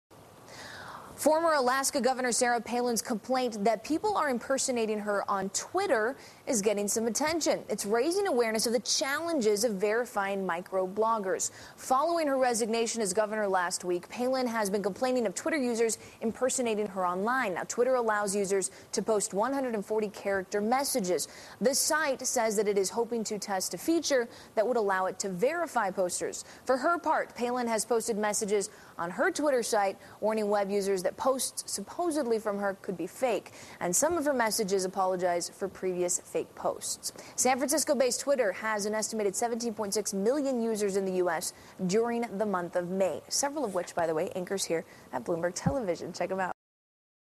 Twitter Impersonators A Problem - Palin - Bloomberg
Former governor Sarah Palin's complaint that people are impersonating her on twitter is getting some attention. (Bloomberg News)